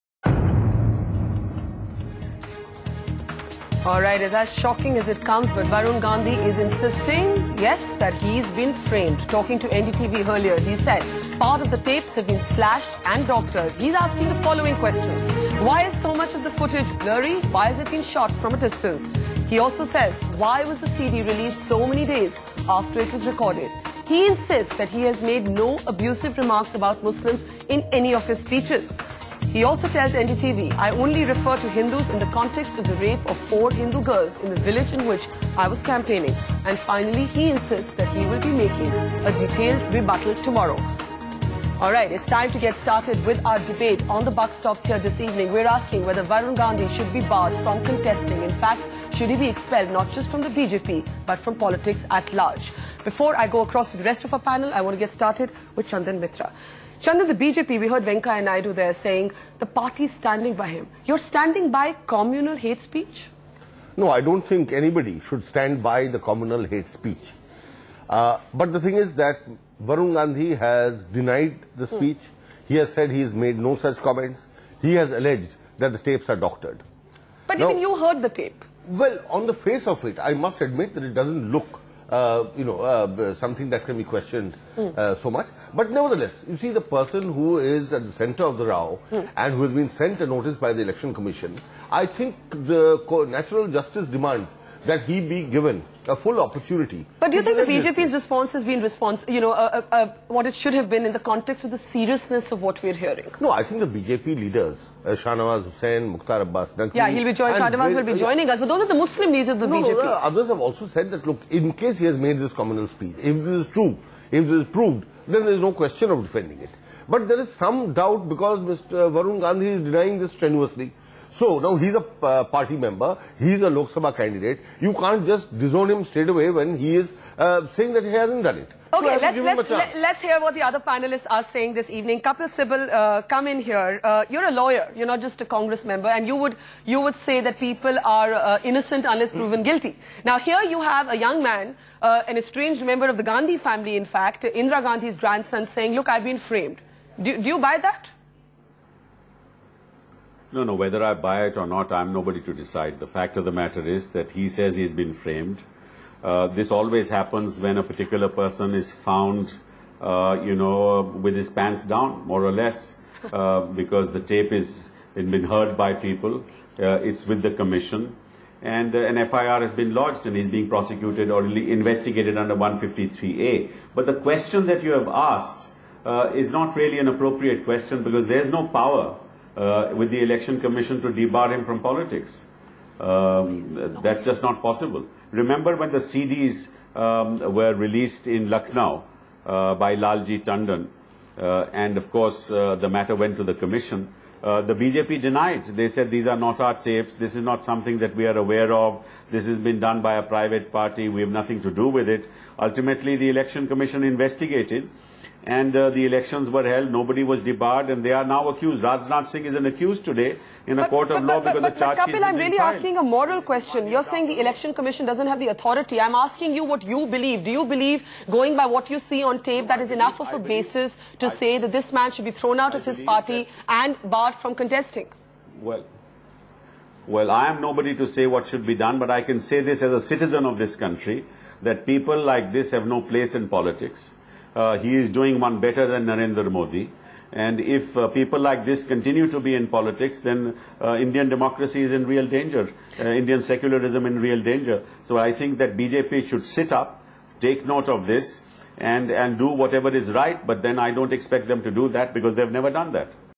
Should Varun Gandhi be expelled from the BJP?
As Varun Gandhi expresses doubts over the tape that shows him making derogatory remarks, we debate if and whether BJP is standing by communal hate speech. Watch the discussion.